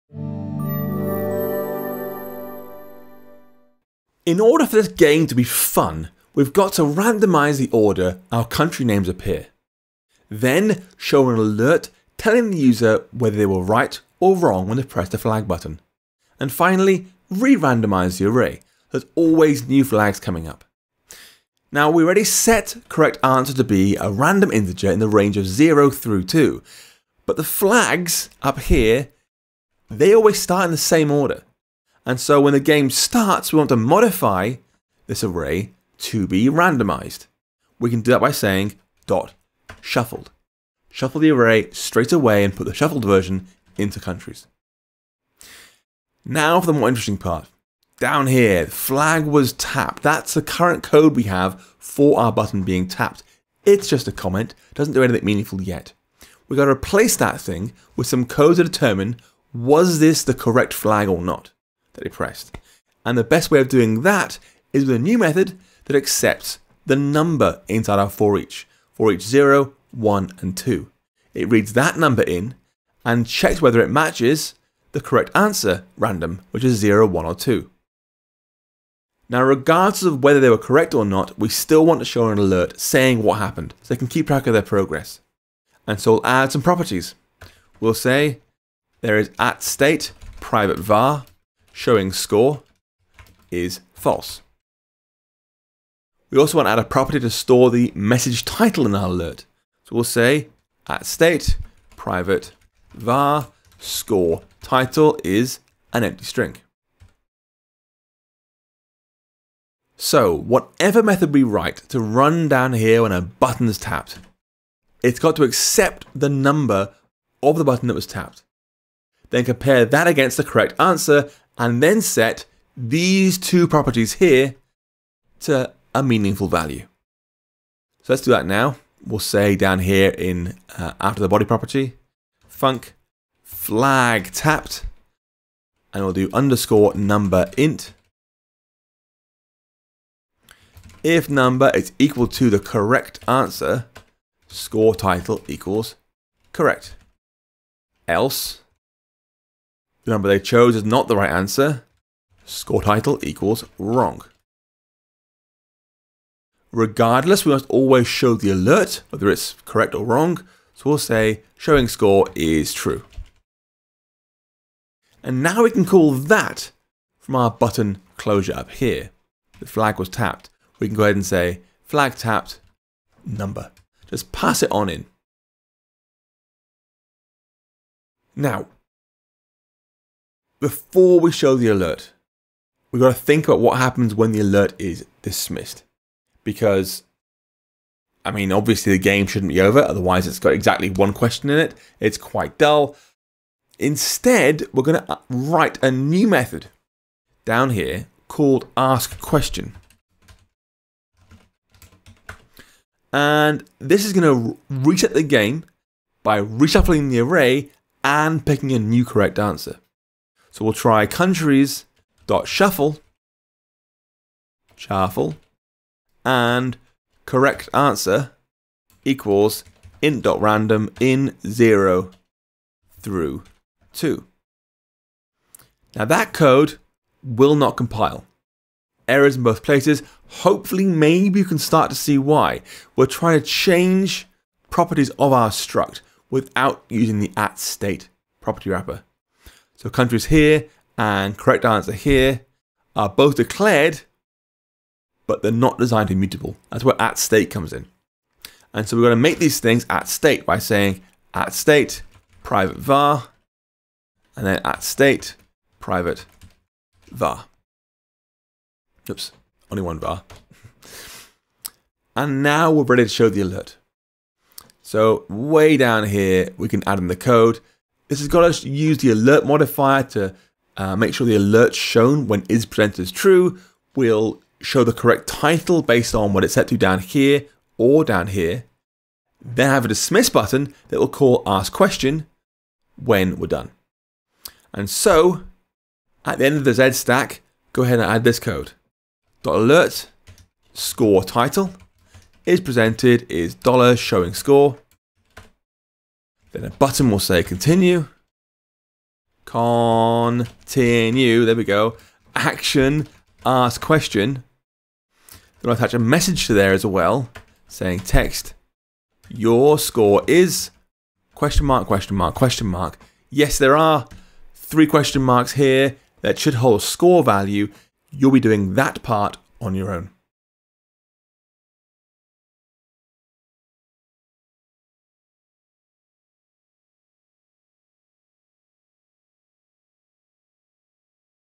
iOS 15: Showing the player’s score with an alert – Guess the Flag SwiftUI Tutorial 7/9
7. Showing the player’s score with an alert: This video